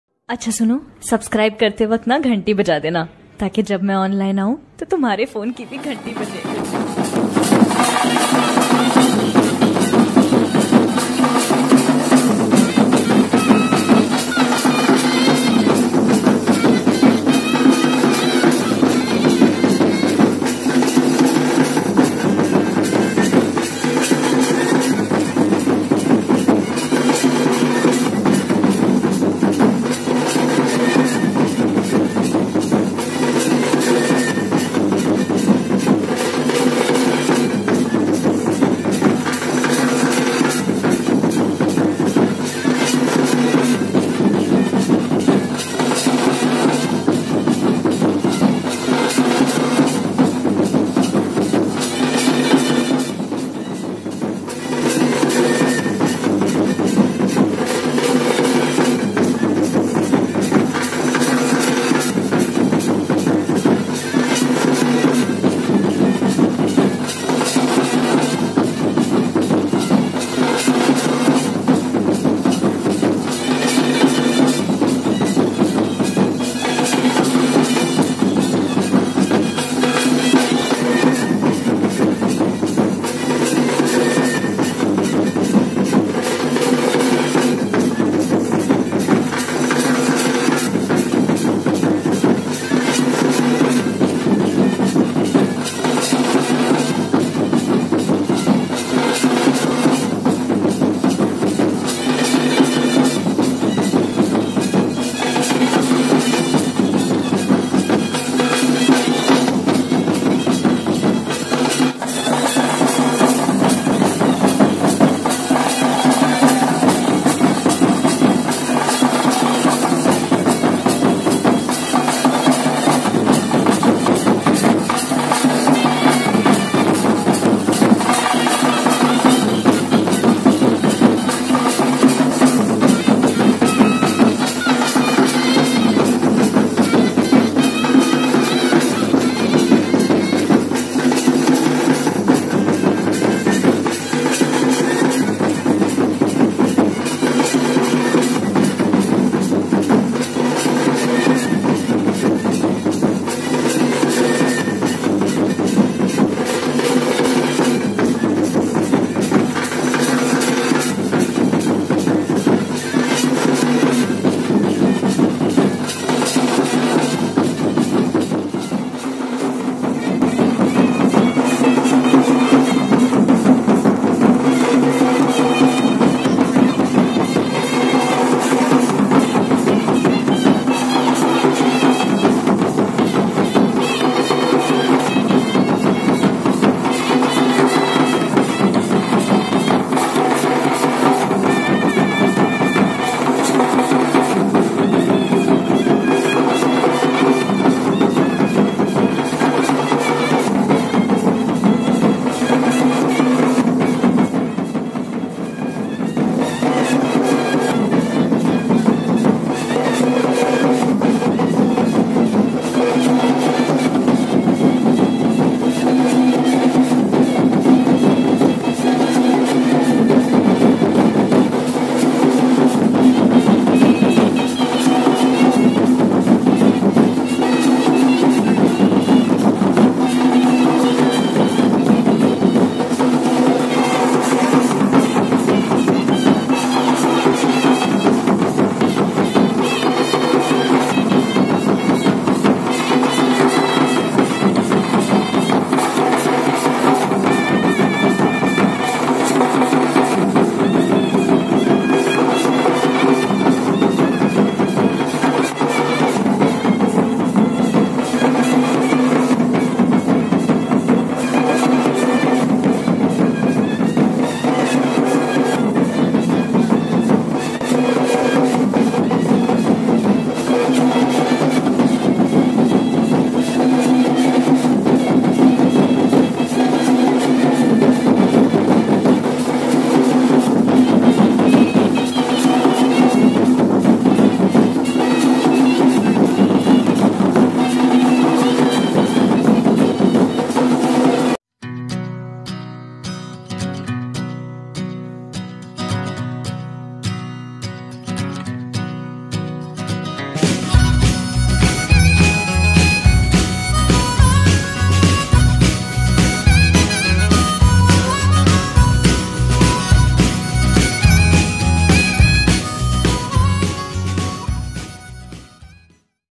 Shadi Chain Dance | Nagpuri Chain Dance | Nagpuri Wedding Dance 2019 | Sailo Dance 💃
पेश है आपके सामने शादी dance 💃रांची  के अंदाज़ में उम्मीद है आपको पसंद आएगा।